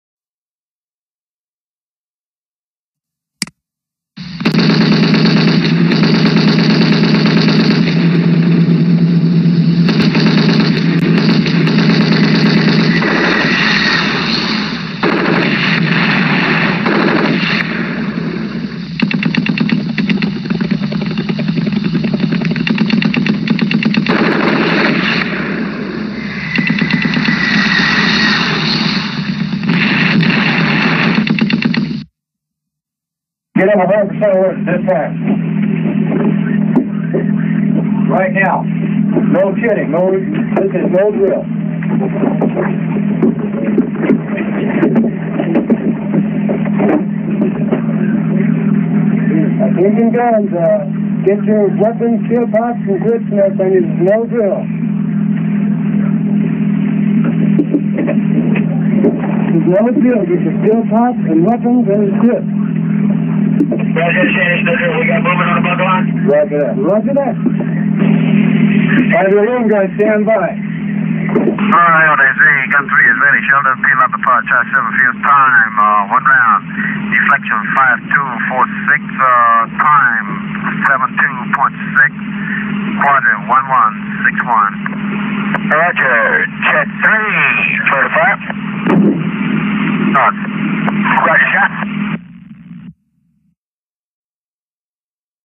Sappers Coming Through The Wire (1955 - 1975)
Chaotic chatter and gun fires recorded during the Vietnam War. It also includes Sappers coming through the wire which in a sapper operation, a small well-trained command attacks a post held by a numerically superior (although still somewhat small) force that is inside the enemy's lines. The Vietnamese called this type of combat the “blooming lotus” tactic—penetrating a fortified area and assaulting outward.

 Field sappers, the largest branch, had two missions: infiltrate U.S. and South Vietnamese installations to destroy targets such as command posts, artillery positions and ammunition dumps; and train conventional infantrymen in perimeter penetration and shock attack.

Credits
Audio